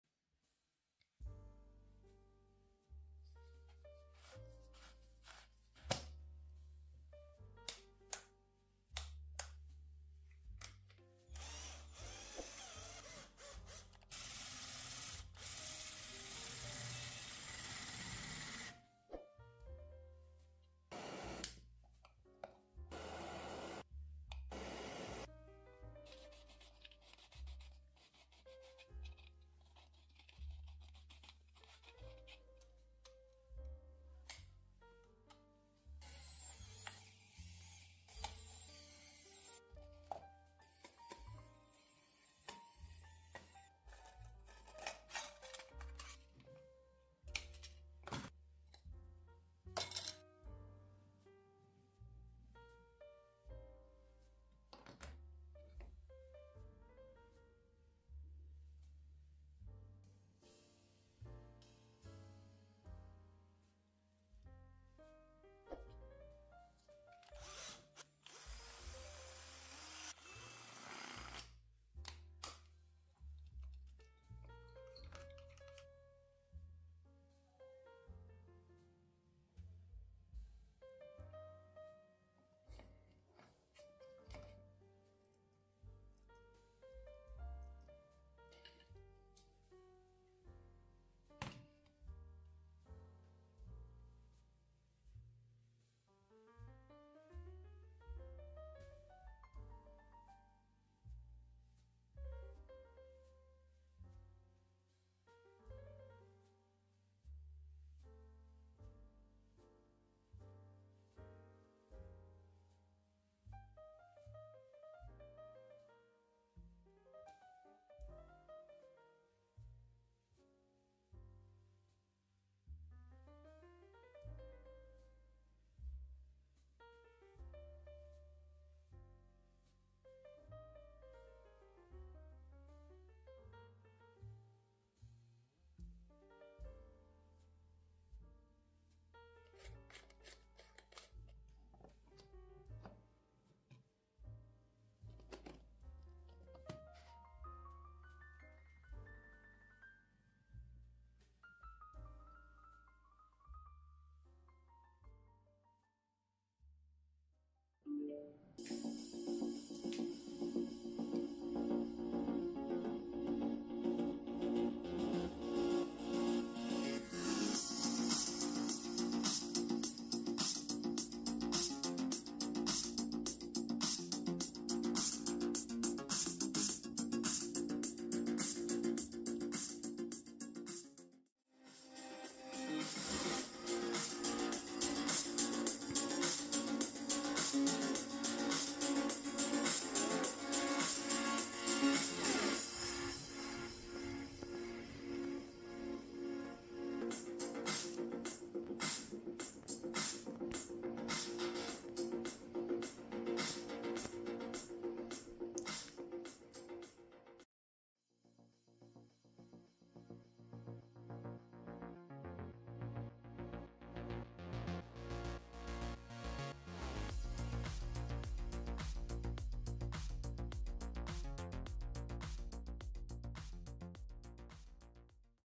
Turn Your Old LED Bulbs into a Bluetooth MP3 Player DIY Guide
Repurpose your old, broken LED bulbs and turn them into a functional Bluetooth MP3 player! In this DIY guide, we show you all the steps to transform old bulbs into a unique project. With this project, you can both recycle and create your own custom MP3 player. Watch the video for the materials you need and step-by-step instructions!

Workshop installation Video:

  • We Established Heavy Industry at Home | To the industry...